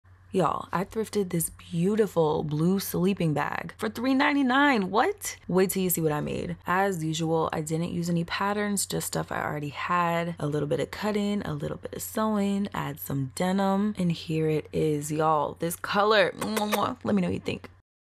Sleeping bag thrift flip 😱
Upcycling a thrifted sleeping bag into a skirt set!